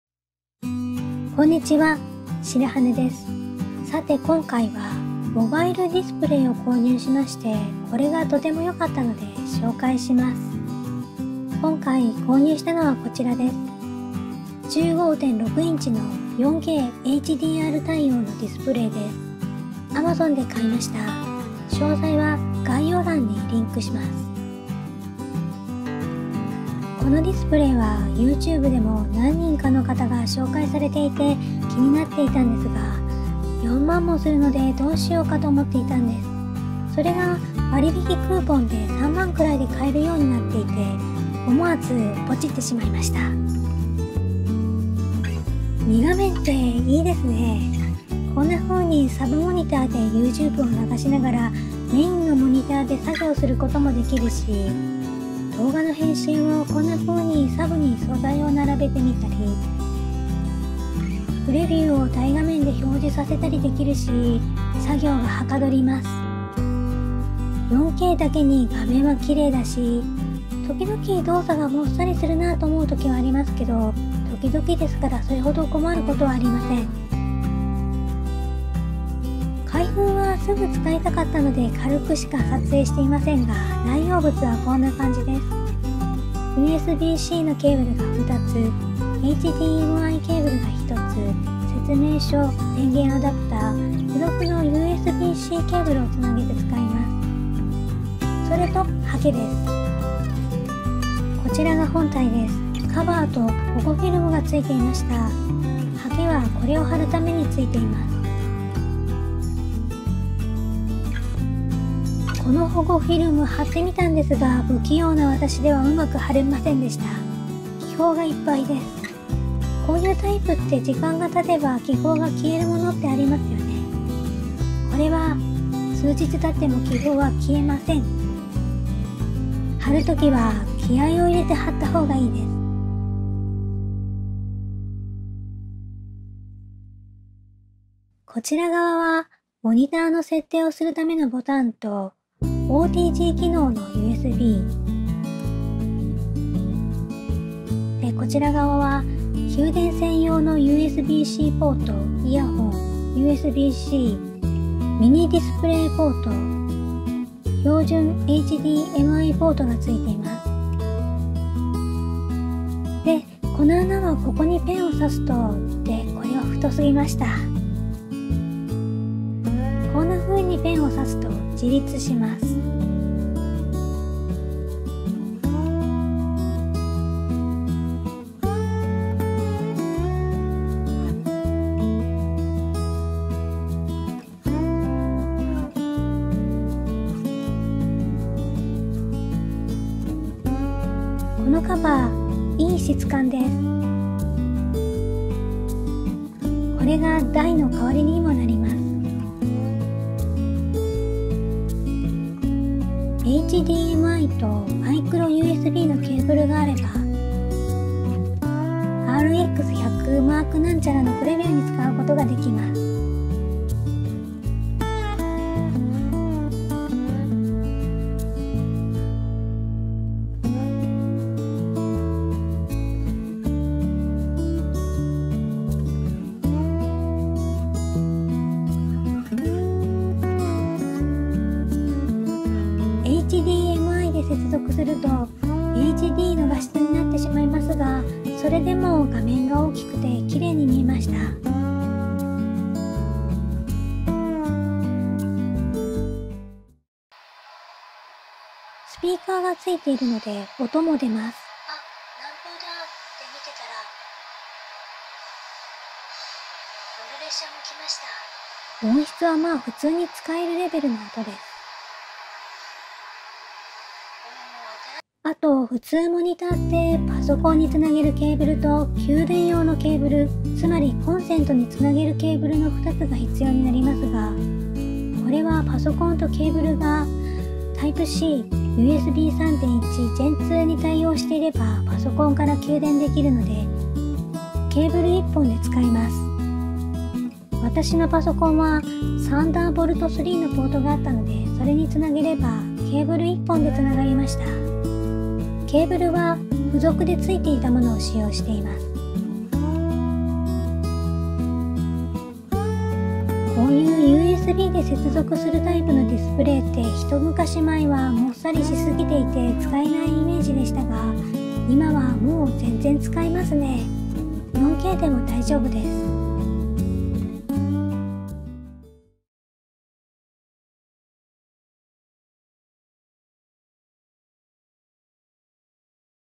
4Kモバイルディスプレイが便利すぎる【 EVICIV EVC-1504】
イマドキのモバイルモニターはケーブルで1本で使えるのに驚きました。
私はゲーム機を持っていないので検証できませんでしたが
ゲーム機とも接続できるらしいです。

今回紹介した製品
モバイルモニター15.6インチ EVICIV 　EVC-1504

Amazonベーシック HDMIケーブル 0.9m